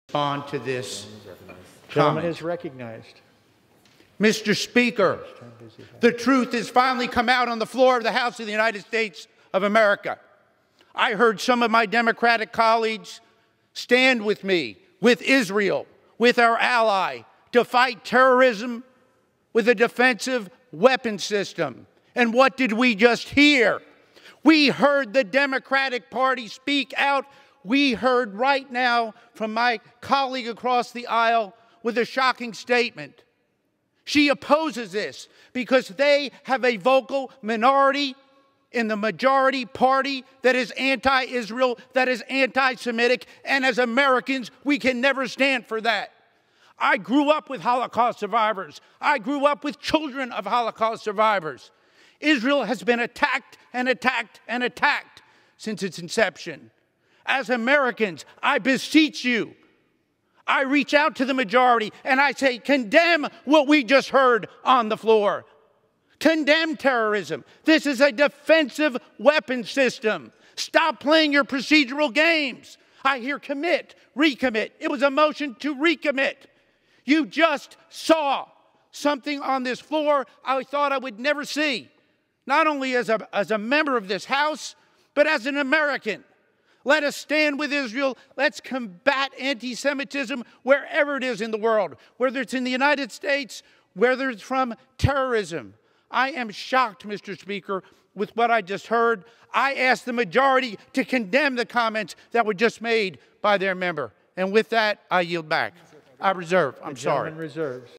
GOP Lawmaker Fires Back At Rashida Tlaib After She Attacks Israel, Calls Her 'Anti-Semitic'
On the House floor, Rep. Charles Fleischmann (R-TN) fired back at Rep. Rashida Tlaib (D-MI) after she accused Israel of war crimes and spoke in opposition of funding Iron Dome.